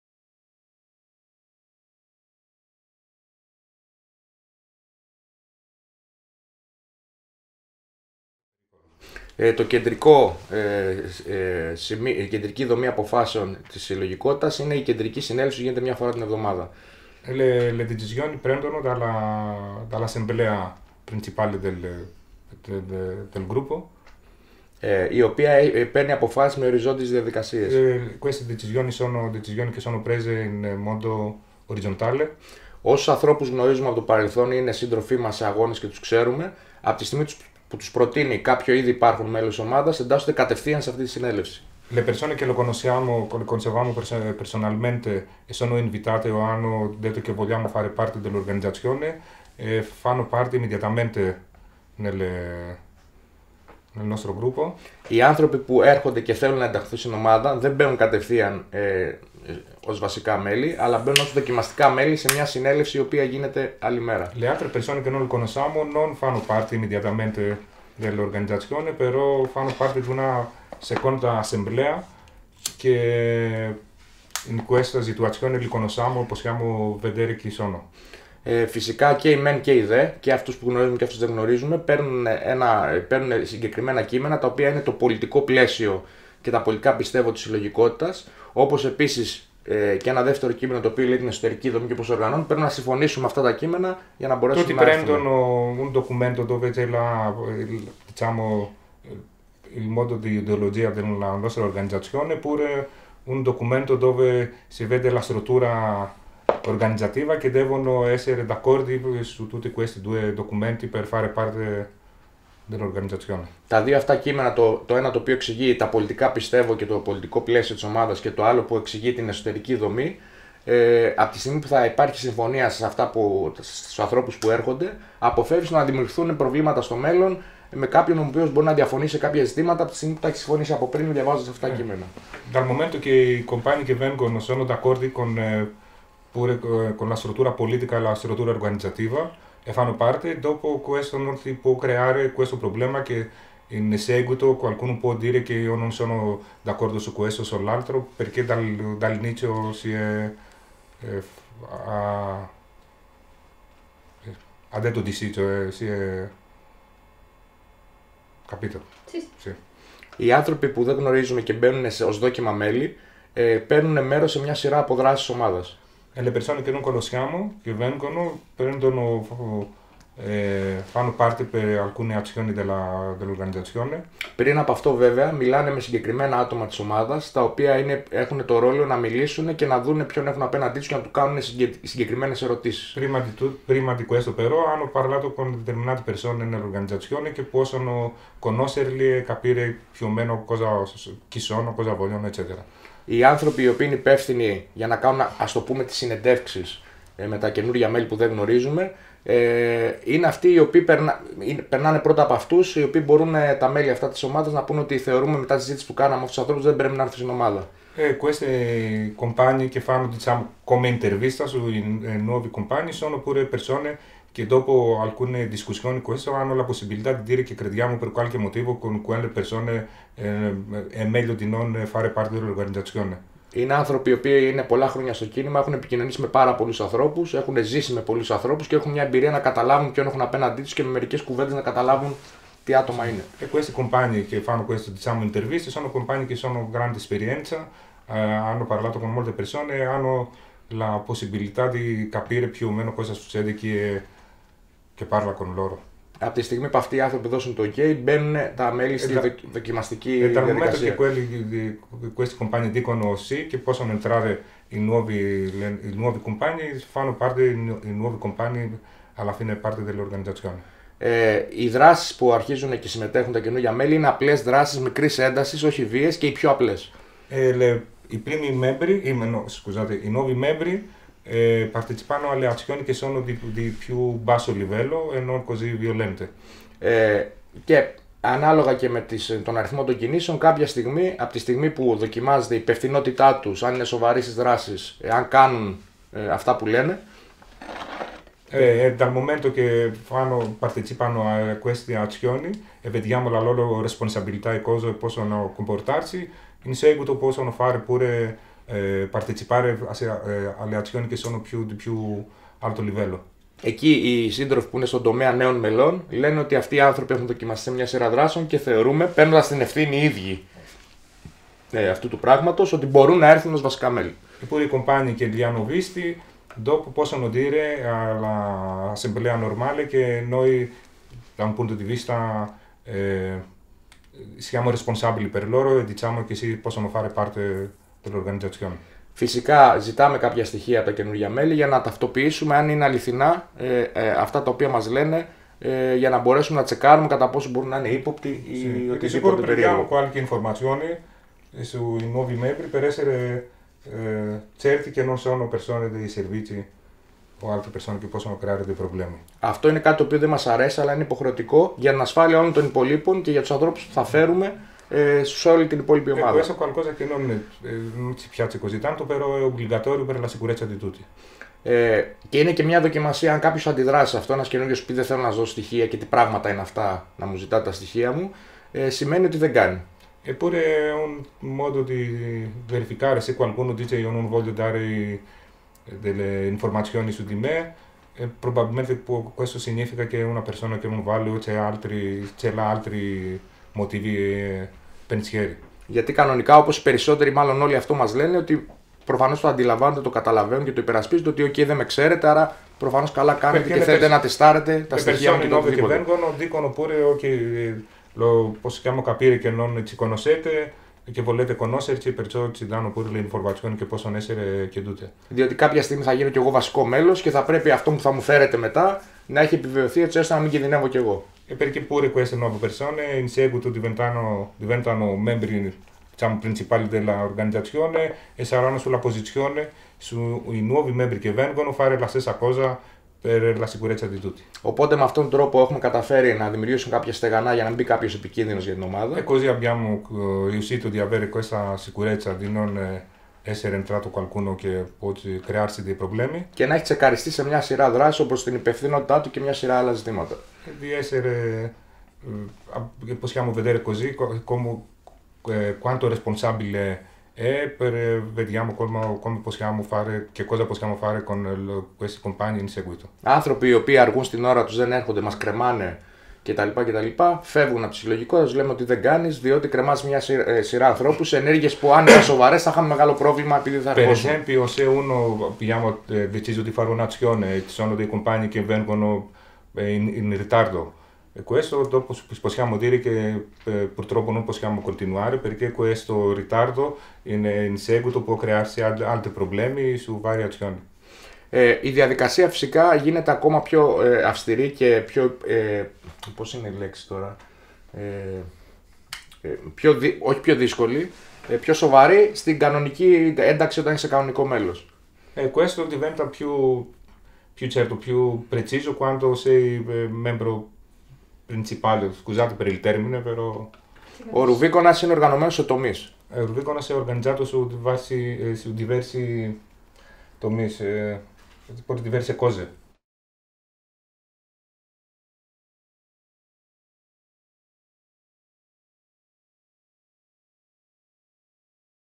ROUVIKONAS - ORGANIZZARSI 2ª parte di 3
ORGANIZZARSI
In occasione della visita in Italia nel gennaio 2019 di due rappresentanti del gruppo anarchico antiautoritario Ρουβίκωνας di Atene, gli abbiamo chiesto di approfondire il tema della ricerca di apertura che praticano per rompere il ripiegamento su se stesso del Movimento per trovare così una nuova efficacia di azione. Buon ascolto!